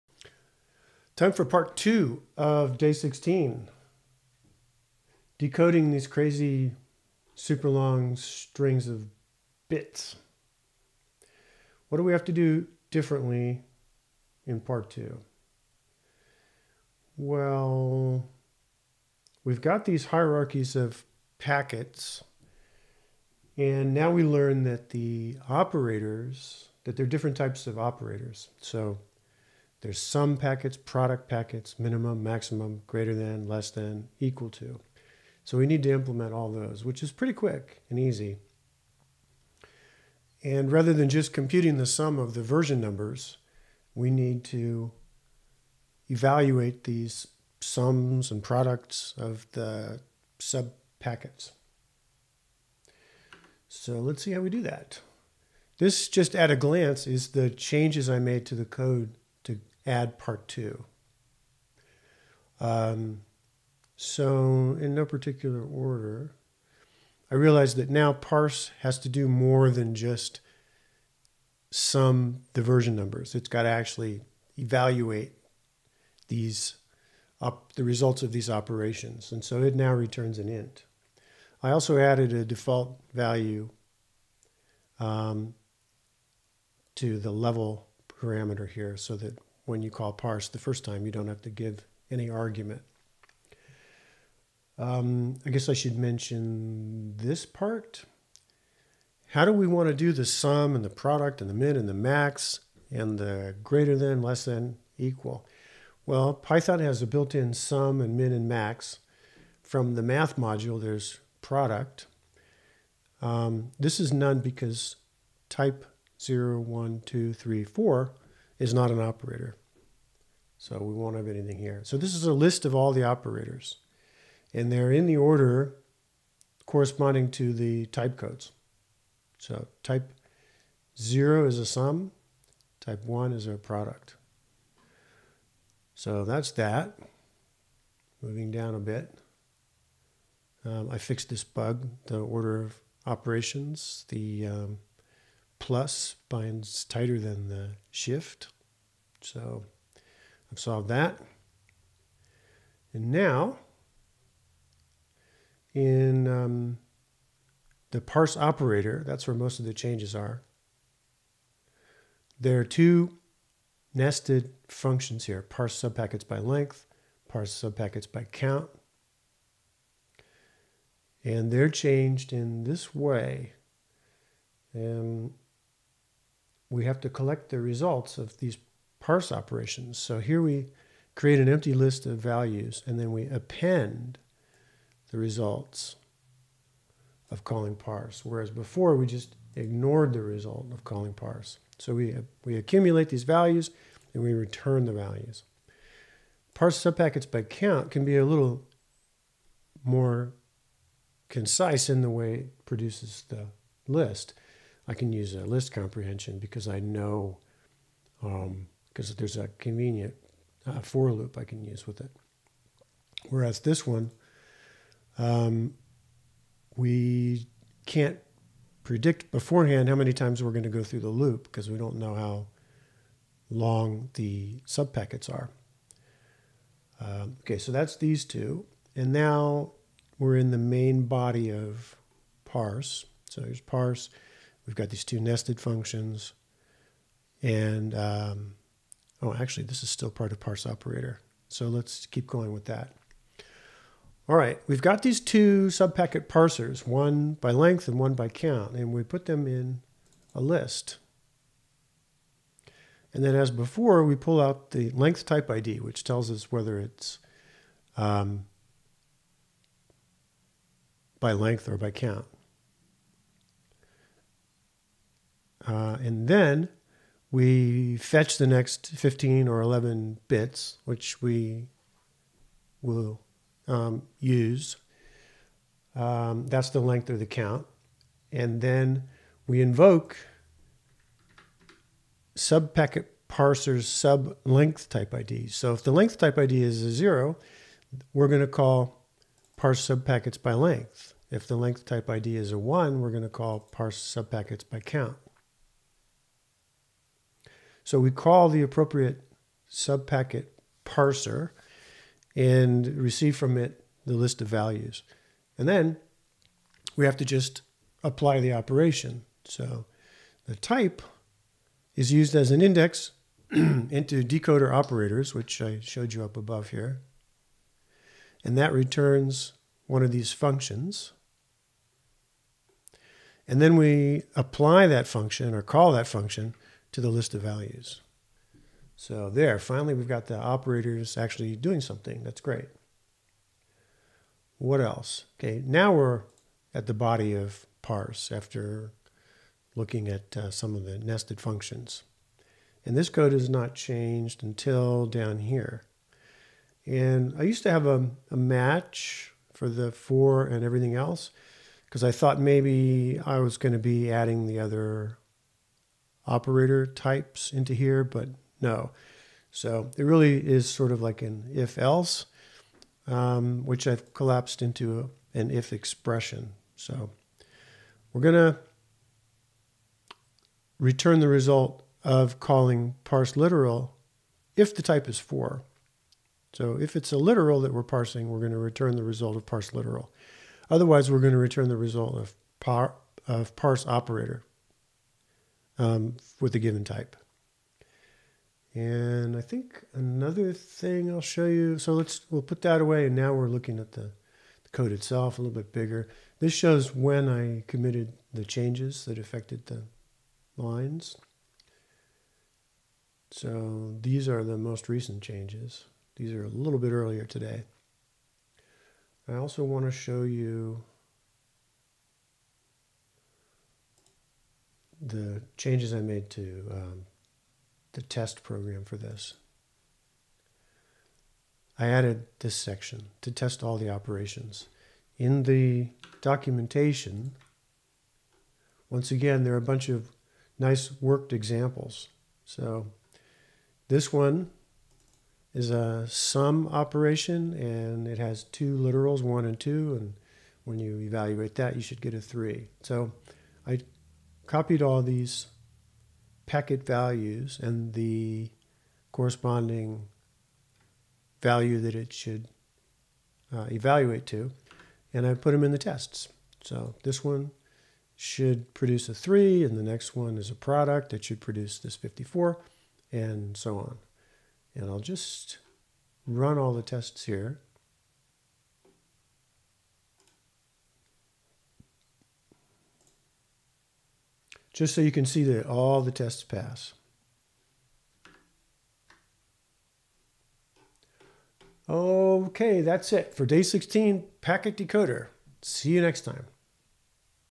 Advent of Code 2021 Day 16, Part 2, Python Solution
Uses unittest.TestCase, and a table-driven operator lookup scheme, to apply the operations sum, product, min, max, etc.